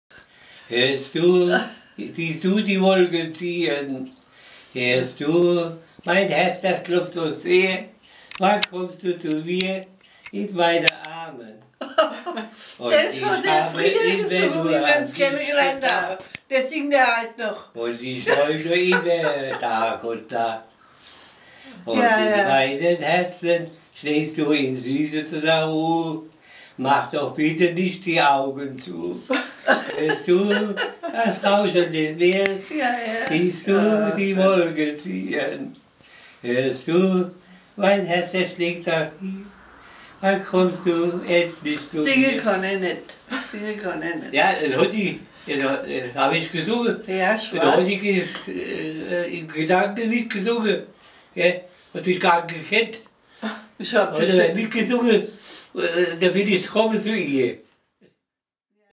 Helmut und Ria Sperber
Das letzte Video vom Keyboardspieler Helmut Sperber und seiner Frau Ria, bevor er gestorben ist. Wir berichteten. Das Video ist spontan entstanden mit einer Fotokamera.
(zurechtgemacht von Christoph Schulte, aufgenommen von Katharina Dubno)